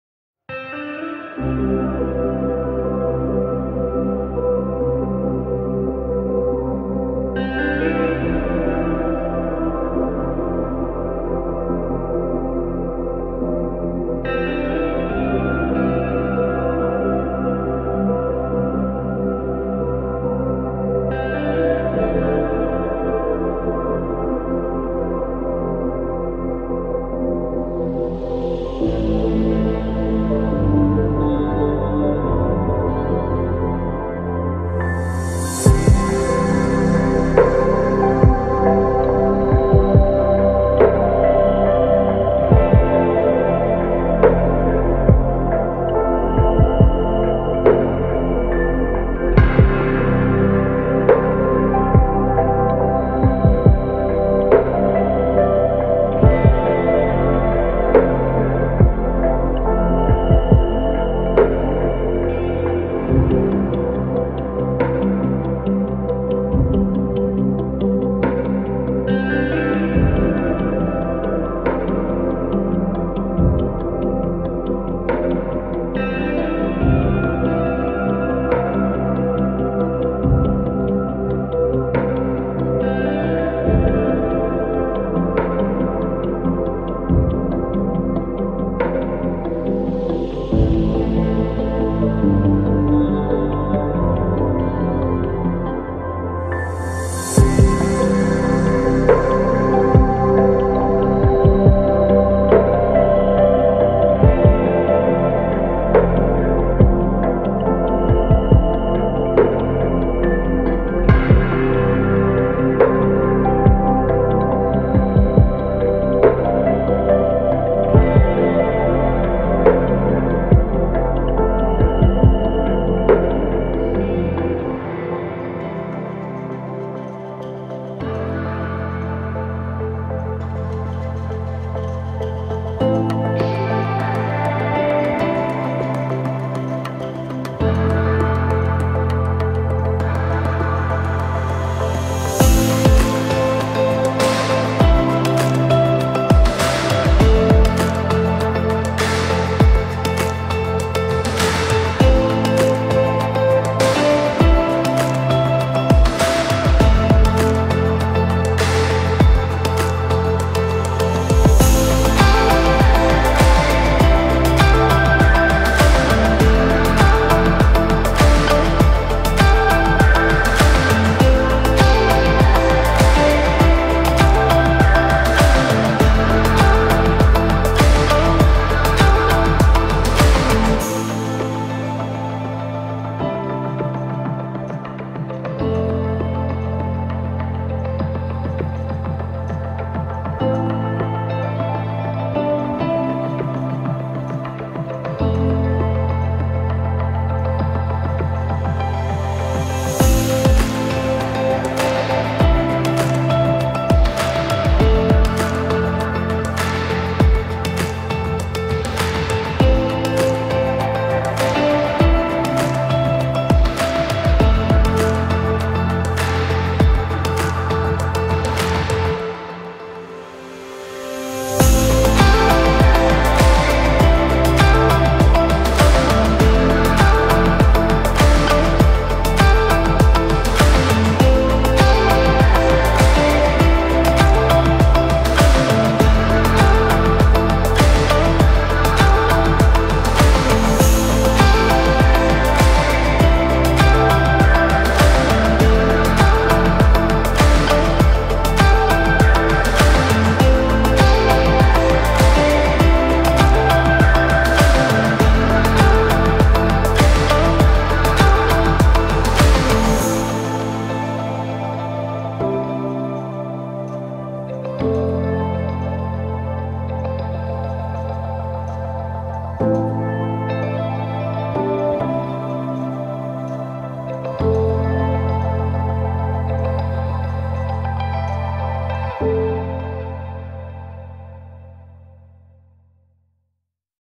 60s time traveller: Asahi Super Takumar 50mm f/1.4
Road tripping with  a cool lens from the 60s:  Asahi Super Takumar (Pentax)  50mm f1.4